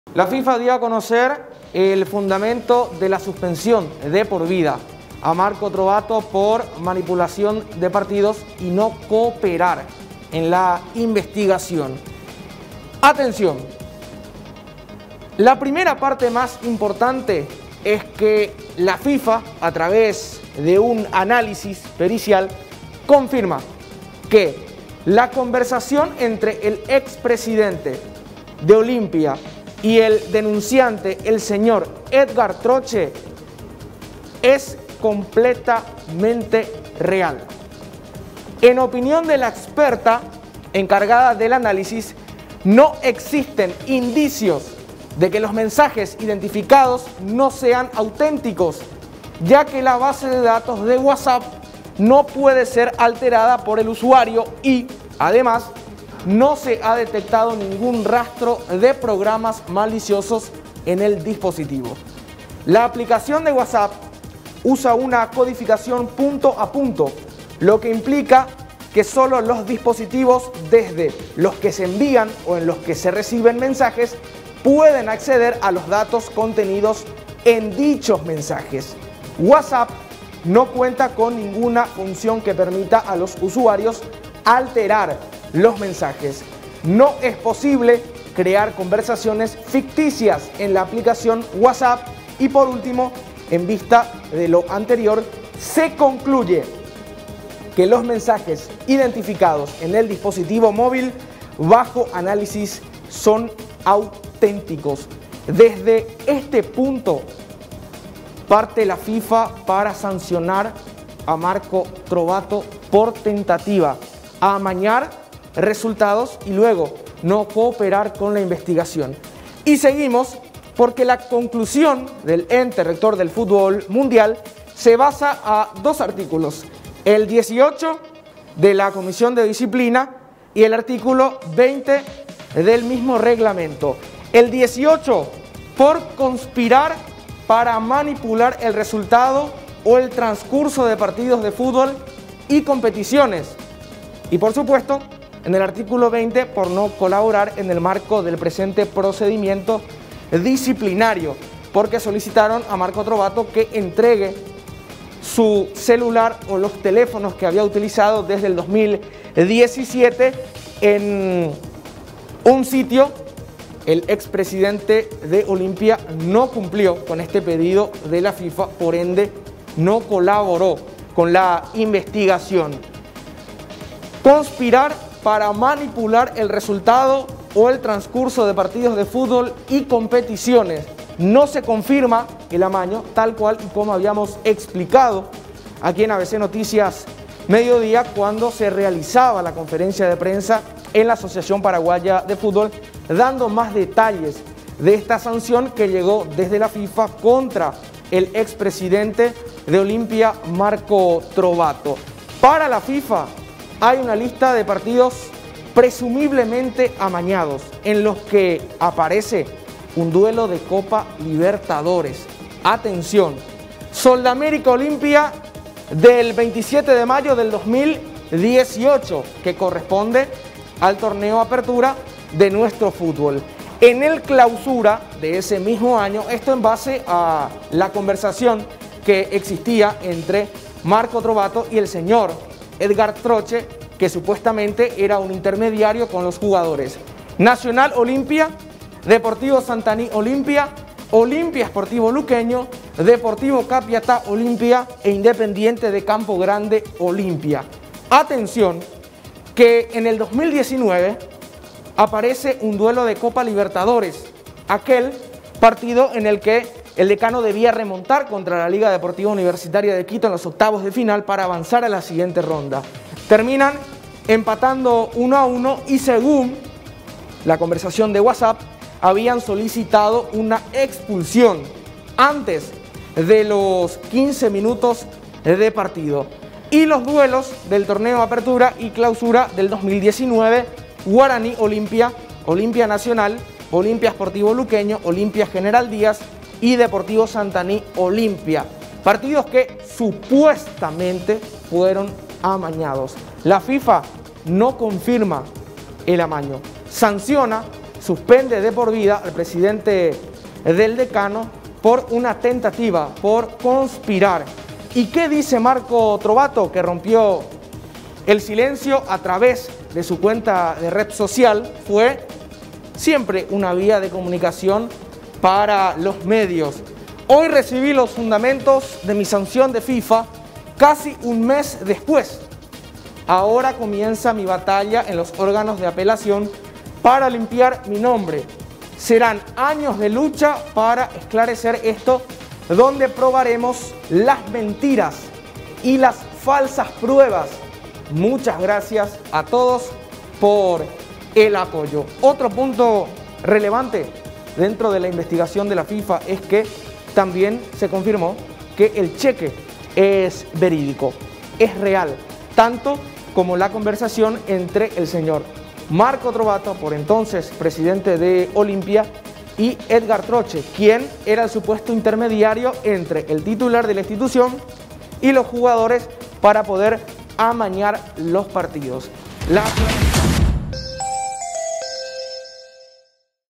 FIFA dio a conocer fundamentos de sanción a Trovato
La Federación Internacional de Fútbol Asociación compartió en su web los detalles sobre el caso que implica al expresidente de Olimpia Marco Trovato. En ella aparecen los partidos presumiblemente afectados.